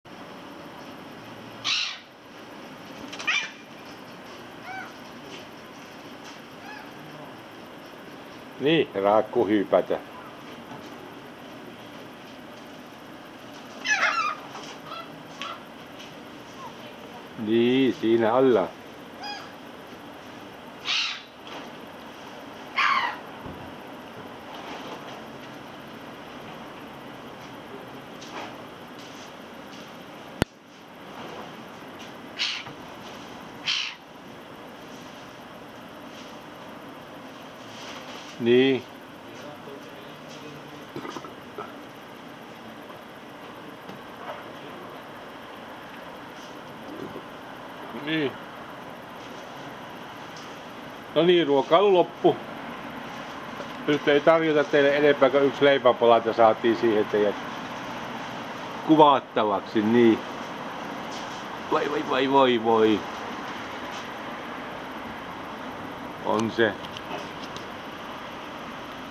Torilla ja Linja auto asemalla on kuhinaa ja elämää nyt lokit on tulleet vierailulle..
Lokit on vierailulla nyt...